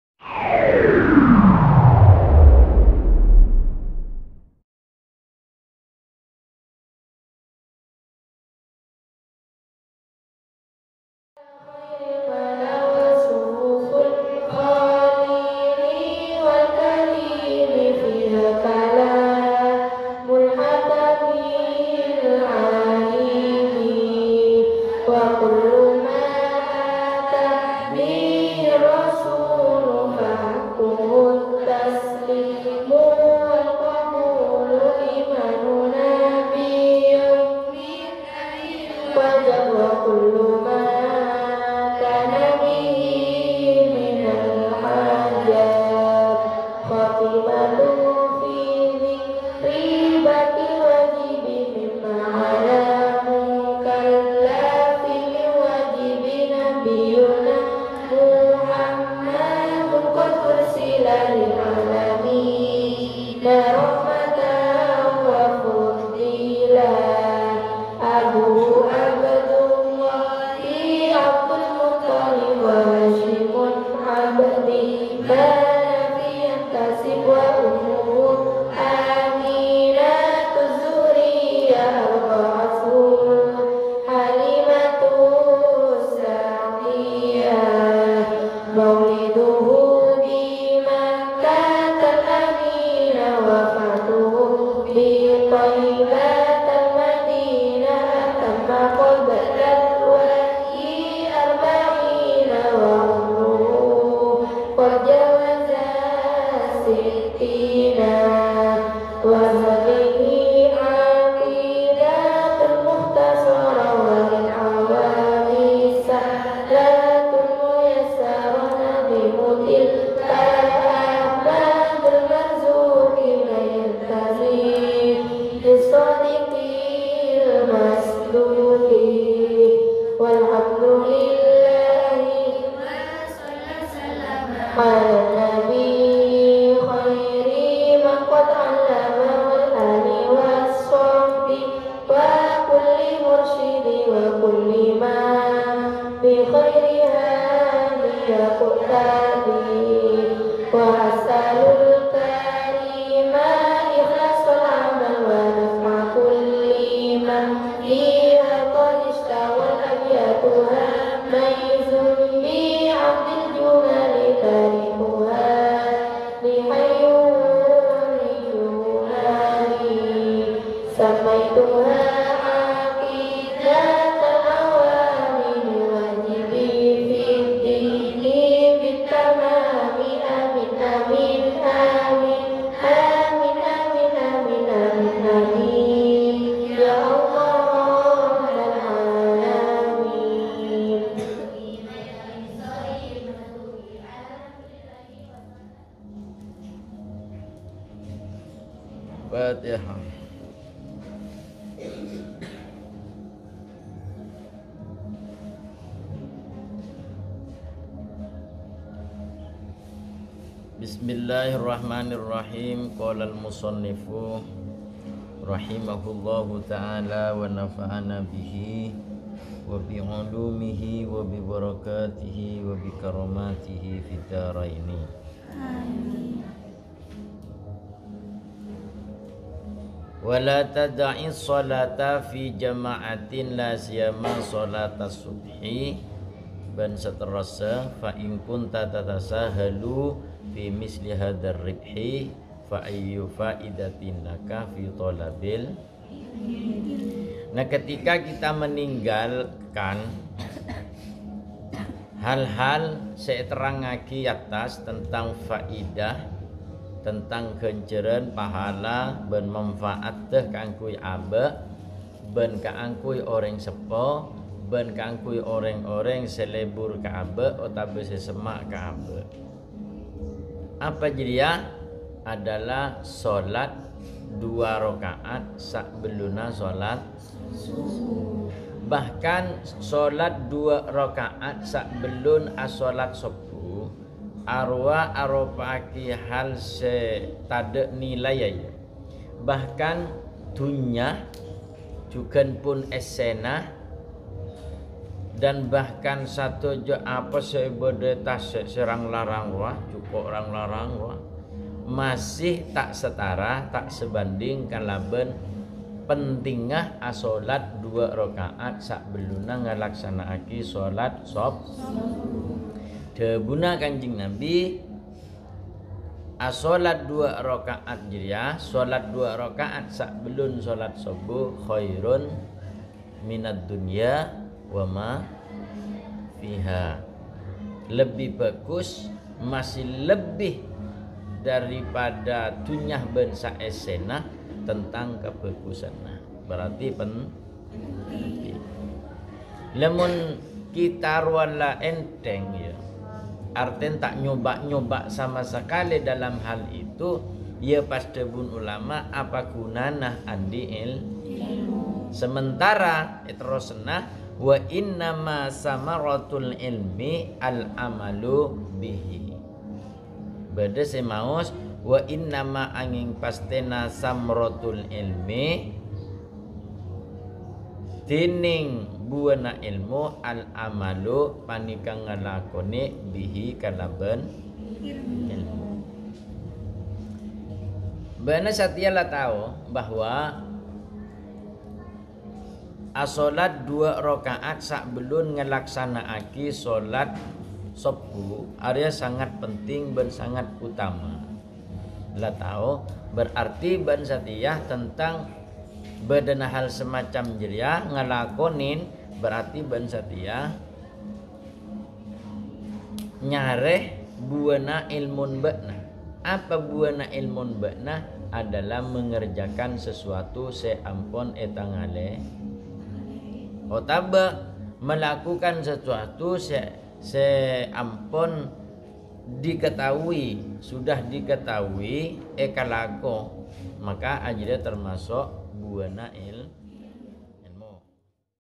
Meninggalkan Penjelasan part 01
Ketika Tidak Memperhatikan Penjelasan Ulam'
Maka Tidak Secara Langsung Di katakan Ilmunya Tidak Mamfaat